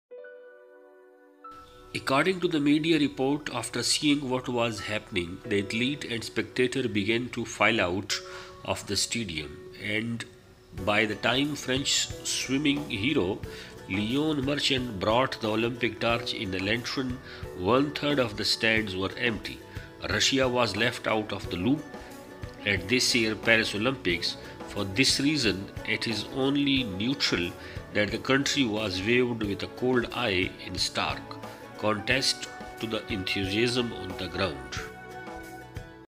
「これは完全な失敗だ」露メディアがパリ五輪の閉会式を酷評！ 世界的スターの出演も内容に不満タラタラ「彼はバイクで早々に…」「３組ともスクリーンの中って」
報道によれば、事態を見た選手や観客は一斉にスタジアムから退場し、フランスの水泳界の英雄レオン・マルシャン選手がランタンに入った聖火を運んだときには、観客席の3分の1が空席になっていたという。今年のパリ五輪ではロシアは蚊帳の外だった。それだけに、現地の熱狂とは裏腹に、ロシアが冷ややかな目で見られていたのも当然だ。構成
"This is a complete failure" - Russian media slams the closing ceremony of the Paris Olympics! Unhappy with the content of the appearance of world-famous stars "He left early on a bike..." "All three groups were on screen"
According to the media report, after seeing what was happening, the athletes and spectators began to file out of the stadium, and by the time French swimming hero Leon Marchand brought the Olympic torch in a lantern, one-third of the stands were empty. Russia was left out of the loop at this year's Paris Olympics. For this reason, it is only natural that the country was viewed with a cold eye, in stark contrast to the enthusiasm on the ground. Compiled by: THE DIGEST Editorial Department.